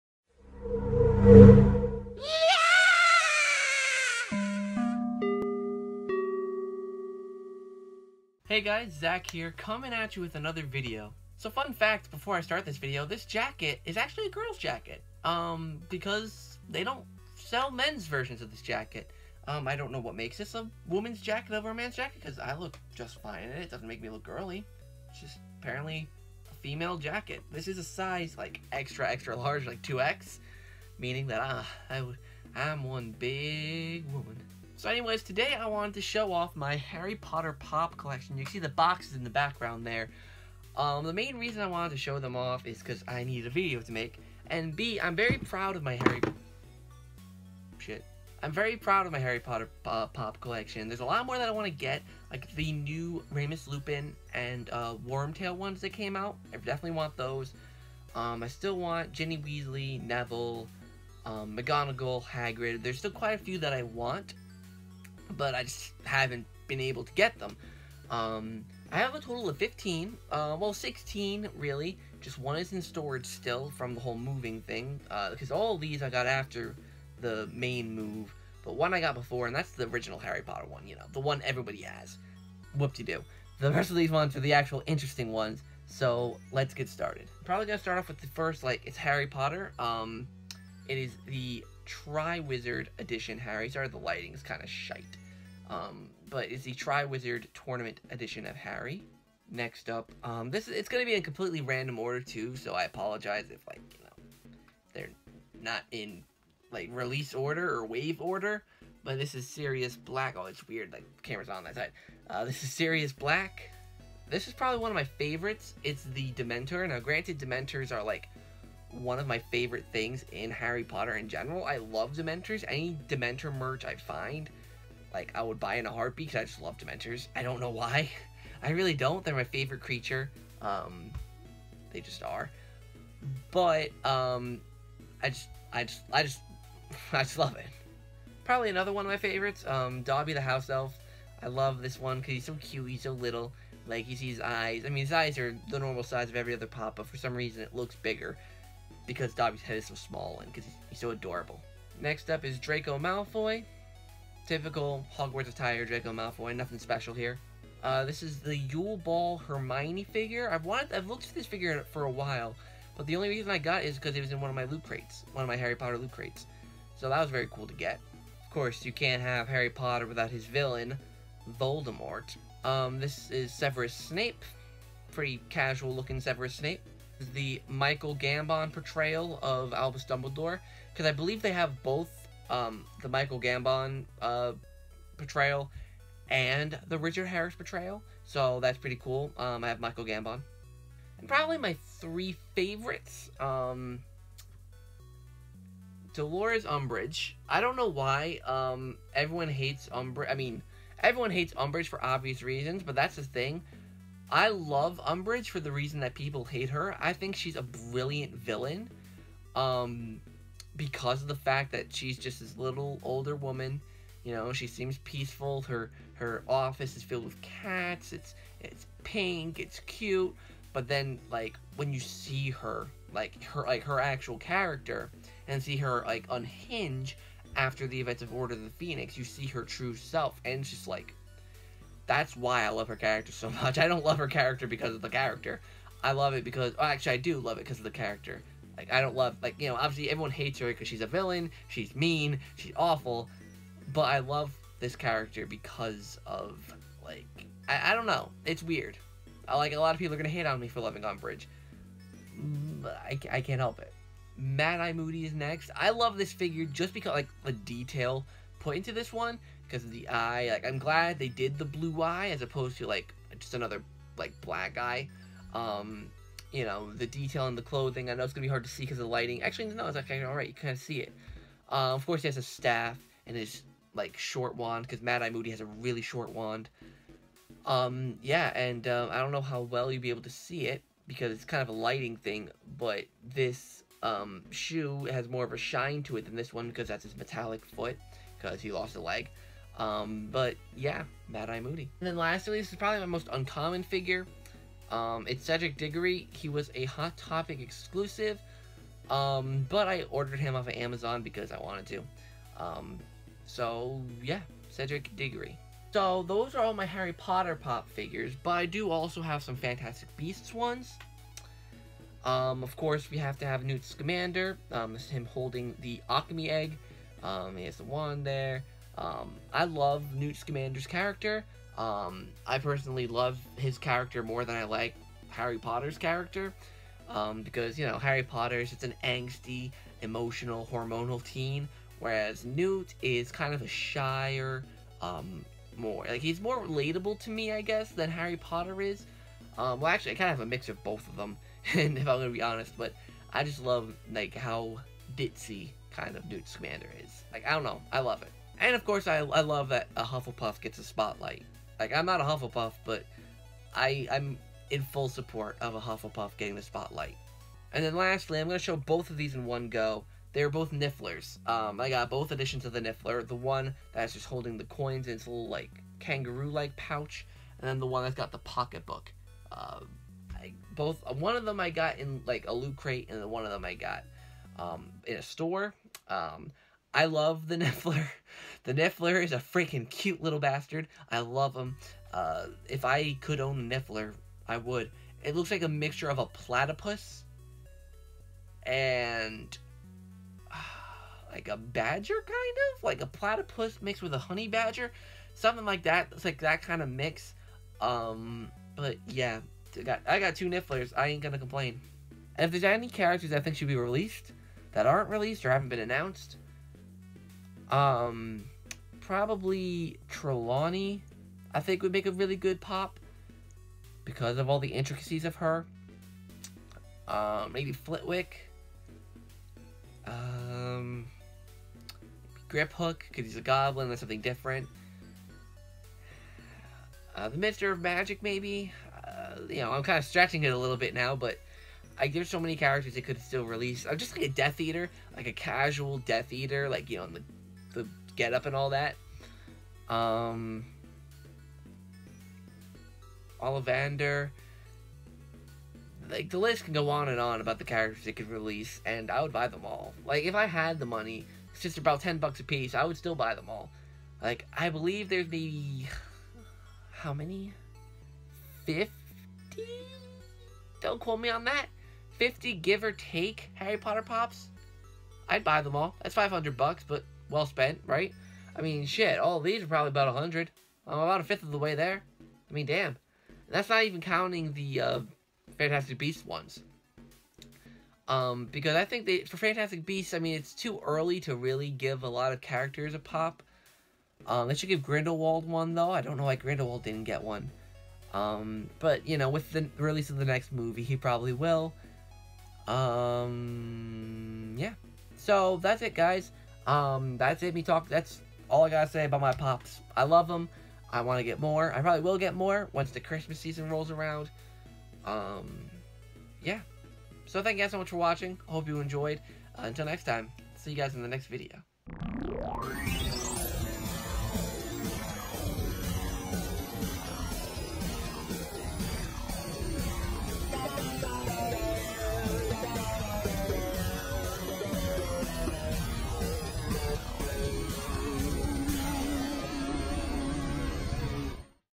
Harry Potter POP! Collection! - ZakPak
Hey Guys!!

This video is showcasing my Harry Potter (and Fantastic Beasts) POP! Vinyl Figures that I have acquired thus far!! I hope you guys will enjoy the video!!